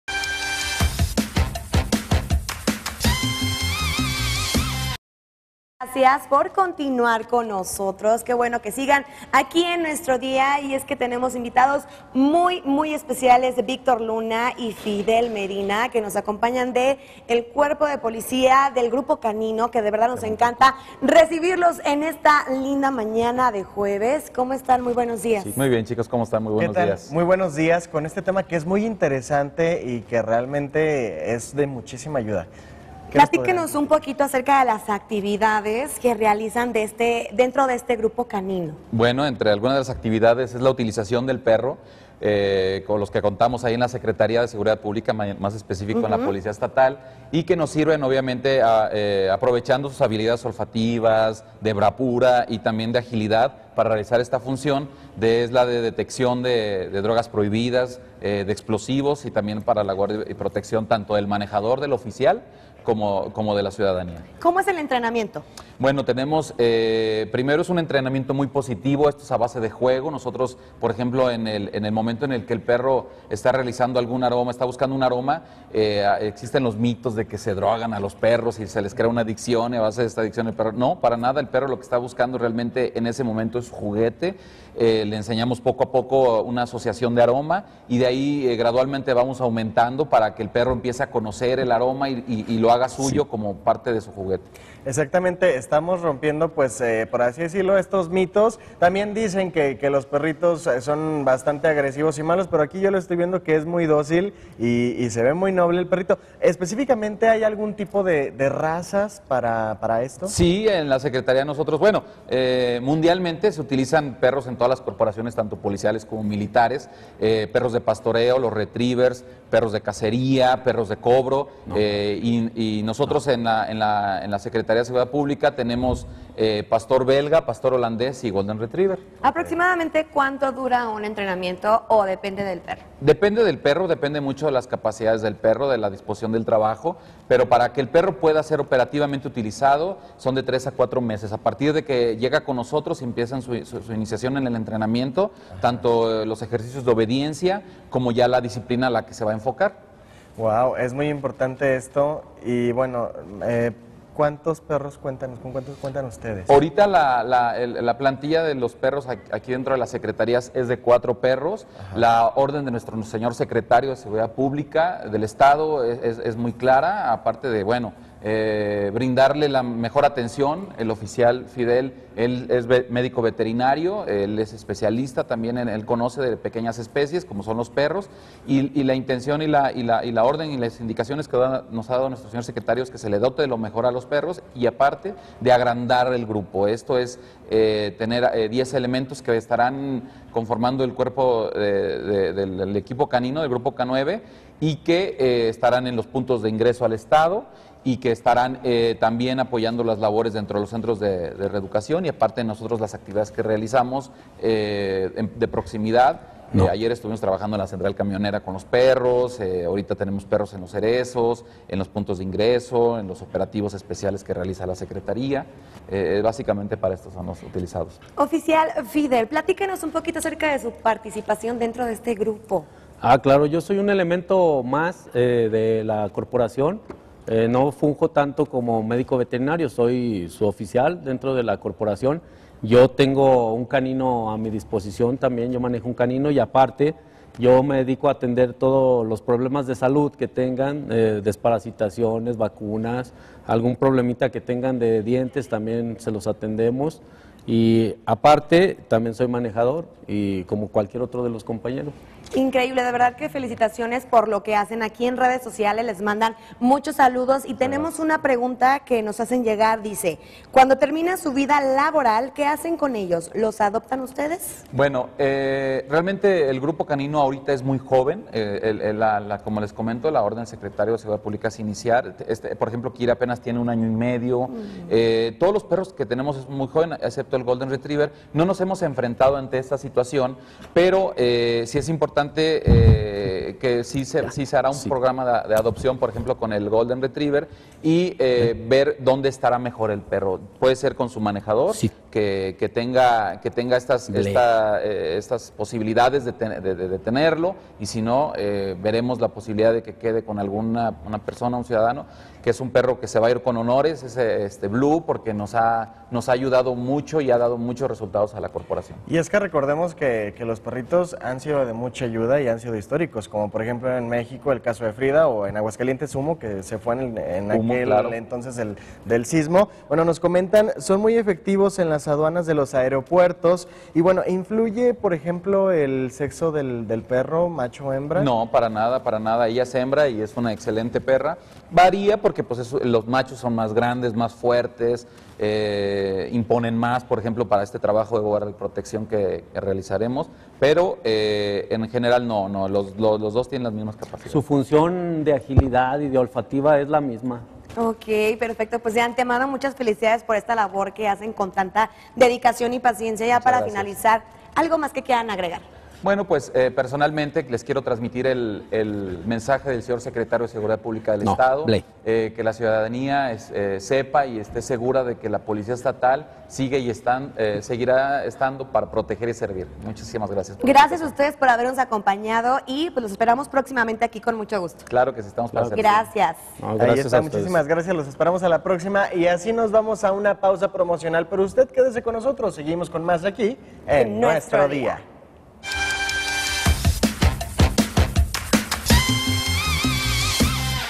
CONOCE A ESTOS PERRITOS POLICÍAS 🐶🐶🐶
El Comandante Víctor Luna y el Oficial Fidel Medina  del Grupo canino de la policía K9 nos presentaron a estos singulares perritos que tienen una gran labor como agentes de la policía.